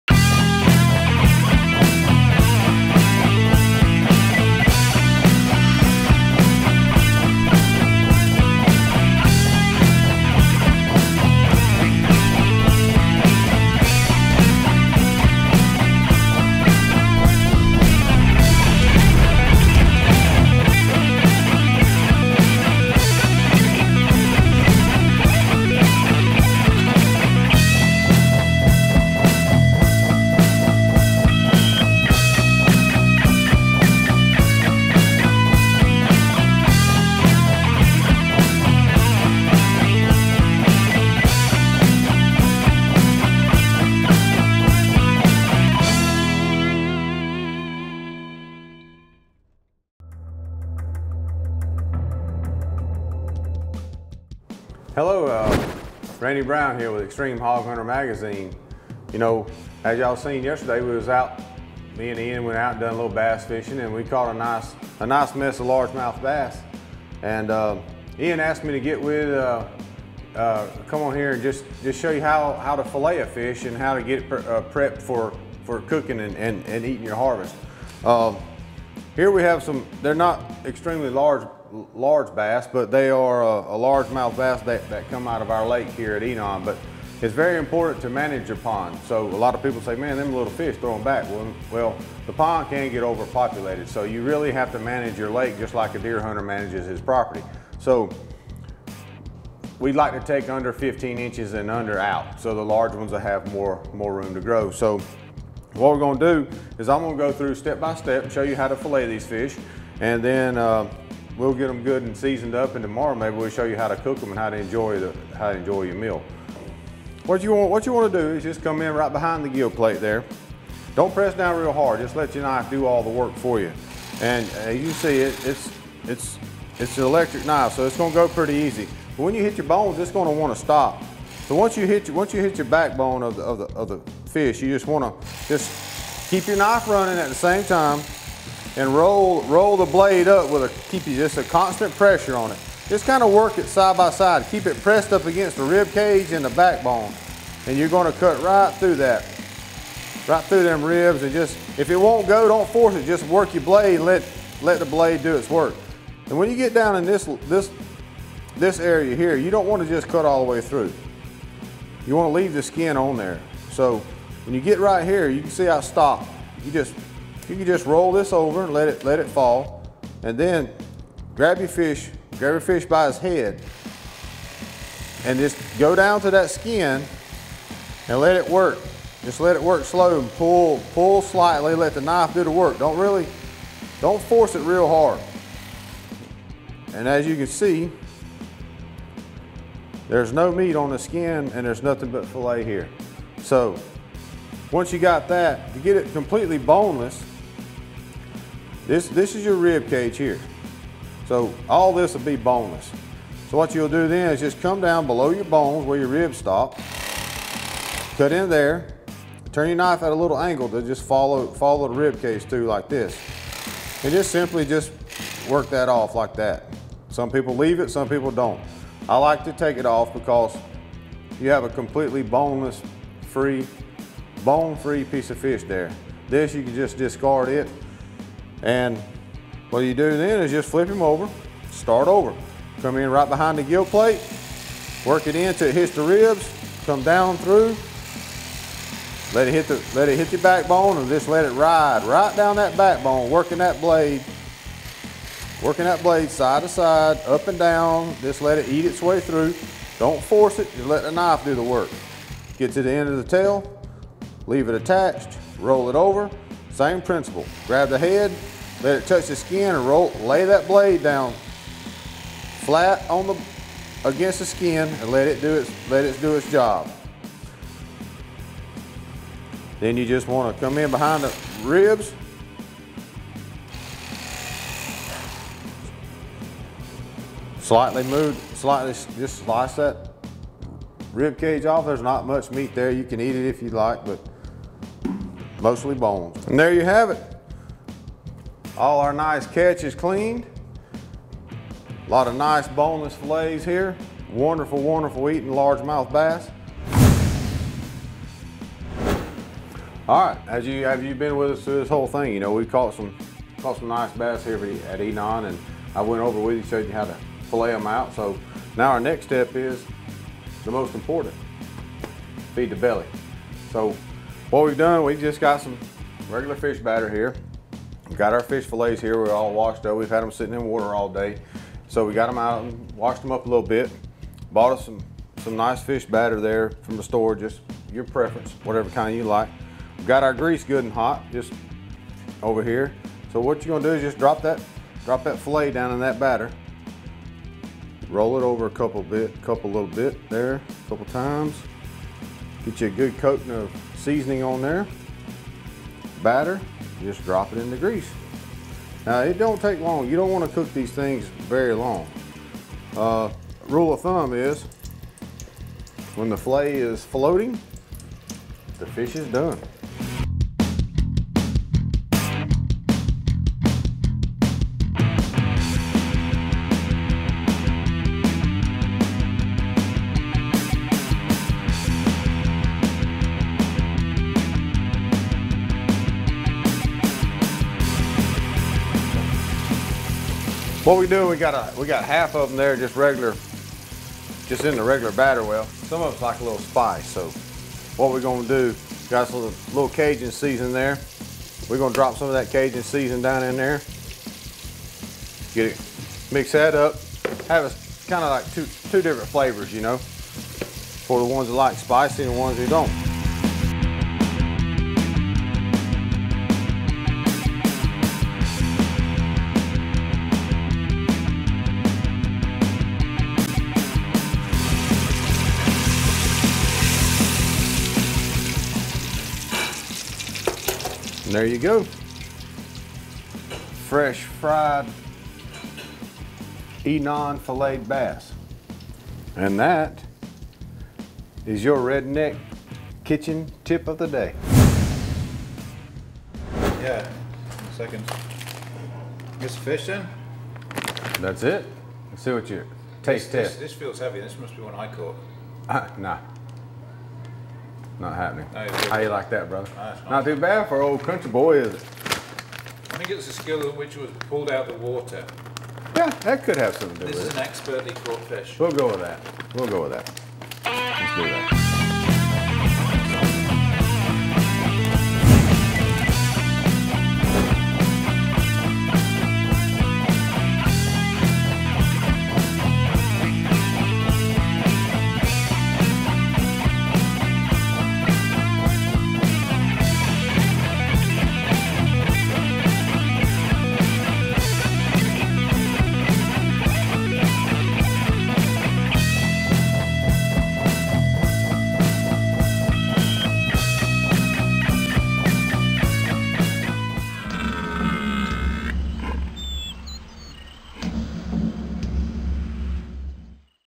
How to Prepare & Cook Bass
Team Wild's Redneck Road Trip: Day 39. We've got a cookery masterclass for you today with hog hunting legend Randy Brown. Having had a successful days fishing on the lake, Randy will now show you how to prep and fillet the bass. Later on, Randy will be cooking his version of fried bass with salad and fries. Enjoy!